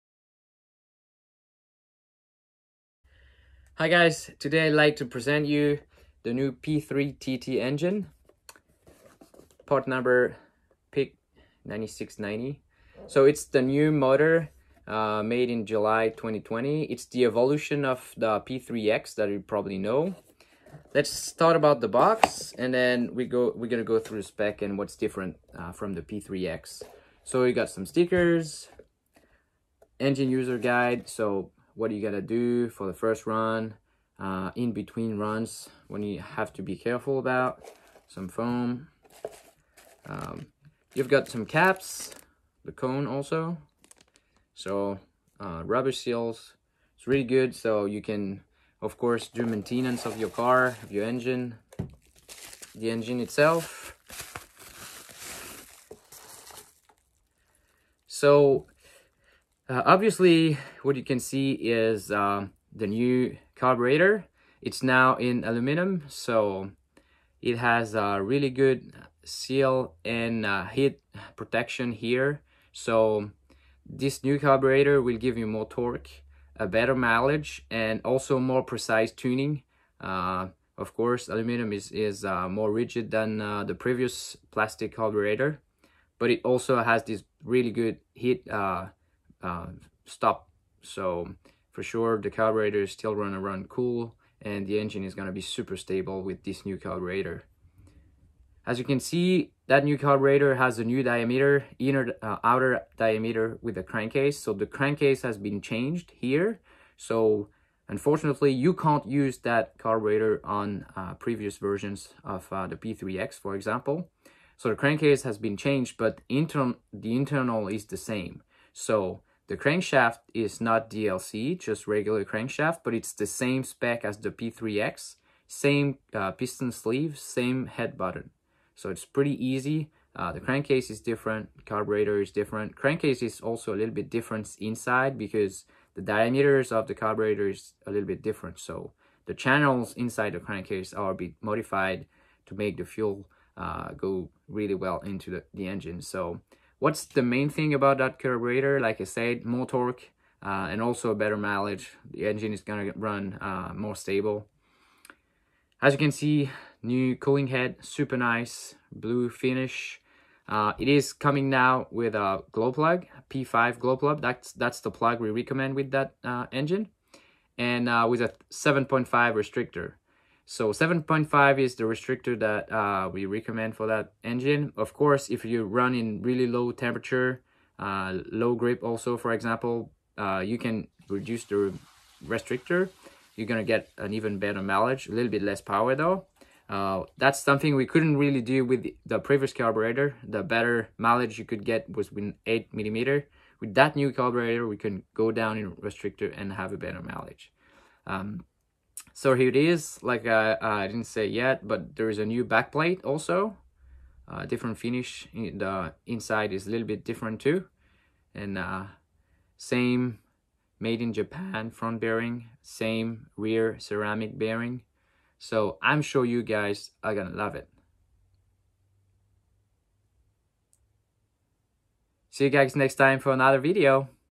Unboxing P3TT
Unboxing and presentation of the latest 1/8 nitro off-road engine from Picco, the P3TT. Released in July 2020, this video will show you all the secrets of the new powerplant from the italian factory.